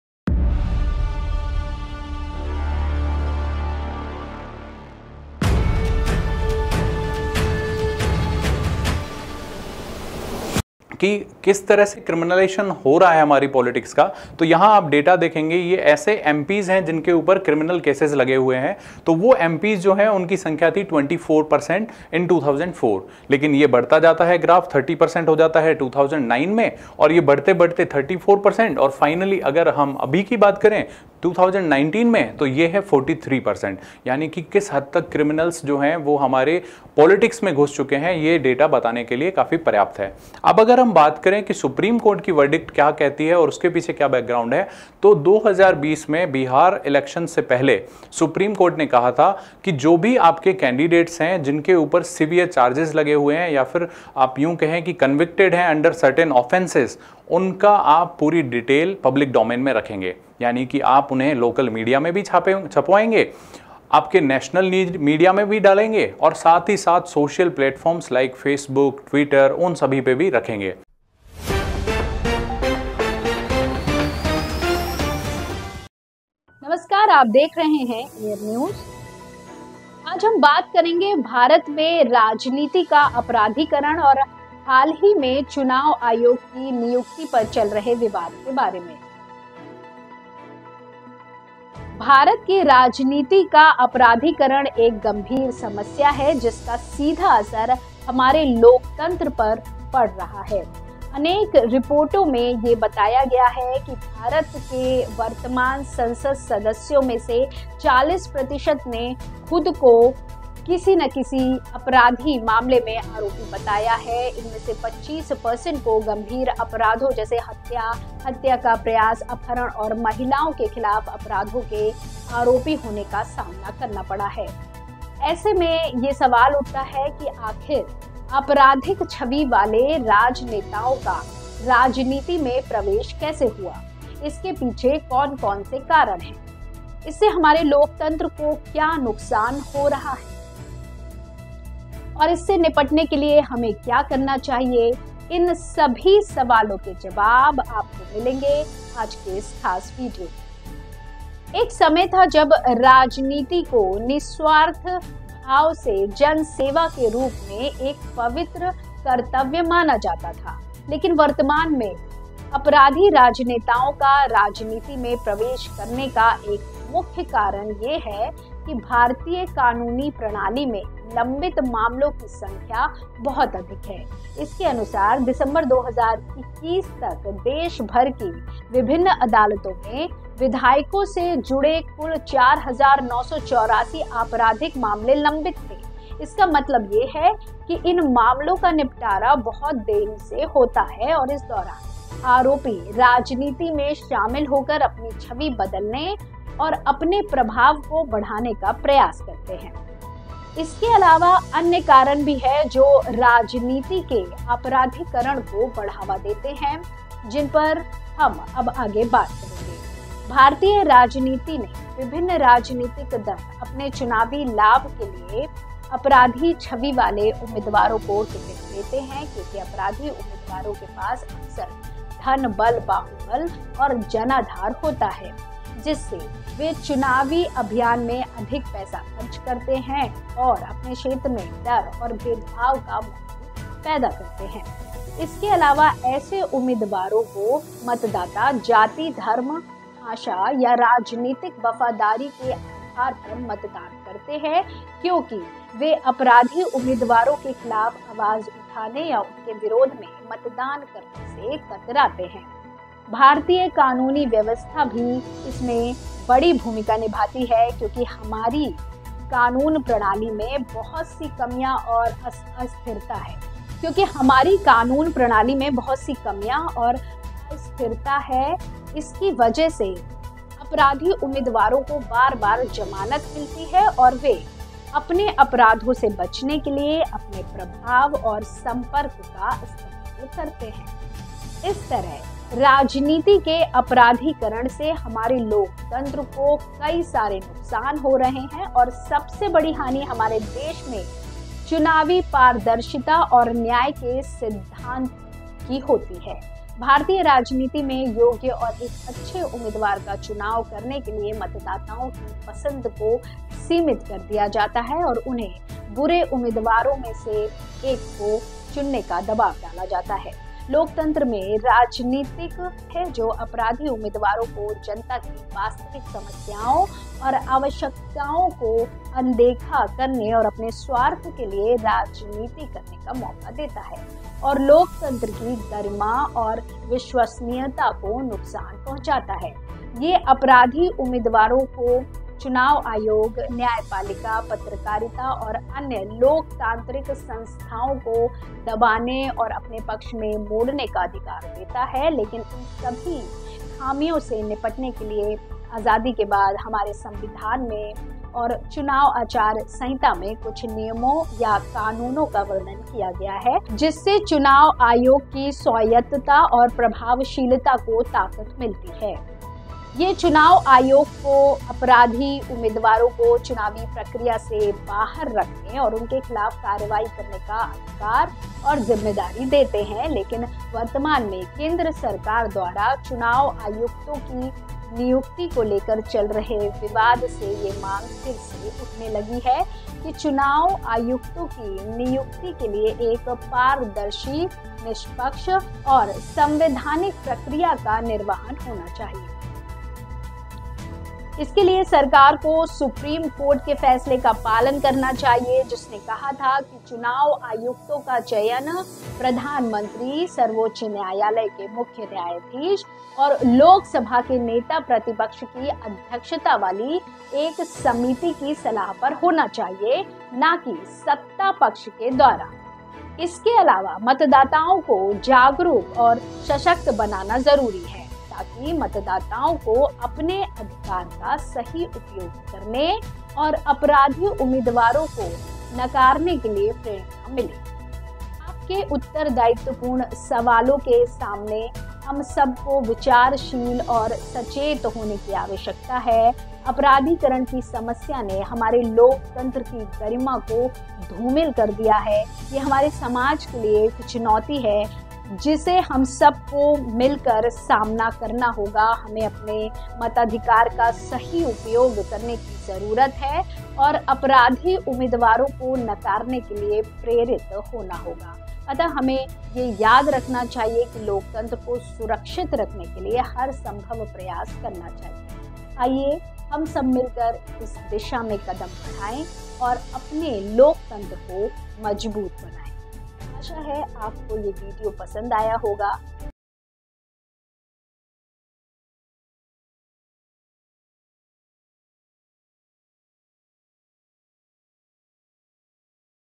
India's Politicized Judiciary: Election Commission Appointment Sparks Criminalization Debate
भारत में राजनीति का अपराधीकरण और चुनाव आयोग की नियुक्ति पर चल रहा विवाद

नमस्कार, आप देख रहे है AIRR न्यूज़।

आज हम बात करेंगे भारत में राजनीति का अपराधीकरण और हाल ही मैं चुनाव आयोग की नियुक्ति पर चल रहे विवाद के बारे में।

भारत की राजनीति का अपराधीकरण एक गंभीर समस्या है, जिसका सीधा असर हमारे लोकतंत्र पर पड़ रहा है। अनेक रिपोर्टों में यह बताया गया है कि भारत के वर्तमान संसद सदस्यों में से 40% ने खुद को किसी न किसी अपराधी मामले में आरोपी बताया है । इनमें से 25% को गंभीर अपराधों जैसे हत्या, हत्या का प्रयास, अपहरण और महिलाओं के खिलाफ अपराधों के आरोपी होने का सामना करना पड़ा है।

ऐसे में ये सवाल उठता है की,
आखिर आपराधिक छवि वाले राजनेताओं का राजनीति में प्रवेश कैसे हुआ?
इसके पीछे कौन-कौन से कारण हैं?
 इससे हमारे लोकतंत्र को क्या नुकसान हो रहा है?
और इससे निपटने के लिए हमें क्या करना चाहिए?
इन सभी सवालों के जवाब आपको मिलेंगे आज की इस खास वीडियो में ।

एक समय था जब राजनीती को निस्वार्थ सेवाभाव से जन सेवा के रूप में एक पवित्र कर्तव्य माना जाता था। लेकिन वर्तमान में अपराधी राजनेताओं का राजनीति में प्रवेश करने का एक मुख्य कारण यह है कि भारतीय कानूनी प्रणाली में लंबित मामलों की संख्या बहुत अधिक है। इसके अनुसार, दिसंबर 2021 तक देश भर की विभिन्न अदालतों में विधायकों से जुड़े कुल 4,984 आपराधिक मामले लंबित थे। इसका मतलब यह है कि इन मामलों का निपटारा बहुत देरी से होता है और इस दौरान आरोपी राजनीति में शामिल होकर अपनी छवि बदलने और अपने प्रभाव को बढ़ाने का प्रयास करते हैं।

इसके अलावा, अन्य कारण भी हैं जो राजनीति के अपराधीकरण को बढ़ावा देते हैं, जिन पर हम अब आगे बात करेंगे
भारतीय राजनीती ने विभिन्न राजनितिक दल अपने चुनावी लाभ के लिए अपराधी छवि वाले उम्मीदवारों को टिकट देते है क्योंकि अपराधी उम्मीदवारों के पास अक्सर धनबल, बाहुबल और जनाधार होता है, जिससे वे चुनावी अभियान में अधिक पैसा खर्च करते हैं और अपने क्षेत्र में डर और भेदभाव का माहौल पैदा करते हैं।

इसके अलावा ऐसे उम्मदवारो को मतदाता जाति, धर्म, भाषा या राजनीतिक वफादारी के आधार पर मतदान करते है क्योंकि वे अपराधी उम्मीदवारों के खिलाफ आवाज उठाने या उनके विरोध में मतदान करने से कतराते हैं।

भारतीय कानूनी व्यवस्था भी इसमें बड़ी भूमिका निभाती है क्योंकि हमारी कानून प्रणाली में बहुत सी कमियाँ और अस्थिरता है । हिस्की वजह से अपराधी उम्मीदवारों को बार-बार जमानत मिलती है और वे अपने अपराधों से बचने के लिए अपने प्रभाव और संपर्क का इस्तेमाल करते हैं।

इस तरह राजनीति के अपराधीकरण से हमारे लोकतंत्र को कई सारे नुकसान हो रहे हैं,और सबसे बड़ी हानि हमारे देश में चुनावी पारदर्शिता और न्याय के सिद्धांत को होती है ।

भारतीय राजनीती में योग्य और एक अच्छे उम्मीदवार का चुनाव करने के लिए मतदाताओं की पसंद को सीमित कर दिया जाता है और उन्हें बुरे उम्मीदवारों में से ही एक को चुनने का दबाव डाला जाता है।

लोकतंत्र में राजनीतिक नीतियों और कार्यक्रमों की कमी भी एक सबसे बड़ी वजह है जो अपराधी उम्मीदवारों को जनता की वास्तविक समस्याओं और आवश्यकताओं को अनदेखा करने और अपने स्वार्थ के लिए राजनीति करने का मौका देता है।

और लोकतंत्र की गरिमा और विश्वसनीयता को नुकसान पहुँचता है । यह अपराधी उम्मीदवारों को चुनाव आयोग, न्यायपालिका, पत्रकारिता और अन्य लोकतांत्रिक संस्थाओं को दबाने और अपने पक्ष में मोड़ने का अधिकार देता है।

लेकिन इन सभी खामियों से निपटने के लिए आज़ादी के बाद हमारे संविधान में और चुनाव अचार सहिंता में कुछ नियमो या कानूनों का वर्णन किया है, जिससे चुनाव आयोग की स्वायत्तता और प्रभावशीलता को ताकत मिलती है । यह चुनाव आयोग को अपराधी उम्मीदवारों को चुनावी प्रक्रिया से बाहर रखने और उनके खिलाफ कार्रवाई करने का अधिकार और जिम्मेदारी देते है।

लेकिन वर्तमान में केंद्र सरकार द्वारा चुनाव आयुक्तों की नियुक्ति को लेकर चल रहे विवाद से ये मांग फिर से उठने लगी है की ,चुनाव आयुक्तों की नियुक्ति के लिए एक पारदर्शी, निष्पक्ष और संवैधानिक प्रक्रिया का निर्वहन होना चाहिए । इसके लिए सरकार को सुप्रीम कोर्ट के फैसले का पालन करना चाहिए, जिसने कहा था कि चुनाव आयुक्तों का चयन प्रधानमंत्री, सर्वोच्च न्यायालय के मुख्य न्यायाधीश और लोकसभा के नेता प्रतिपक्ष की अध्यक्षता वाली एक समिति की सलाह पर होना चाहिए। ना की सत्ता पक्ष के द्वारा।
इसके अलावा मतदाताओं को जागरूक और सशक्त बनाना जरुरी है , ताकि मतदाताओं को अपने मताधिकार का सही उपयोग करने और अपराधी उम्मीदवारों को नकारने के लिए प्रेरणा मिले ।

आपके उत्तर्दायित्वपूर्ण सवालों के सामने हम सबको विचारशील और सचेत होने की आवश्यकता है। अपराधीकरण की समस्या ने हमारे लोकतंत्र की गरिमा को धूमिल कर दिया है। यह हमारे समाज के लिए एक चुनौती है जिसे हमें मिलकर सामना करना होगा। हमें अपने मताधिकार का सही उपयोग करने की जरूरत है और अपराधी उम्मीदवारों को नकारने के लिए प्रेरित होना होगा।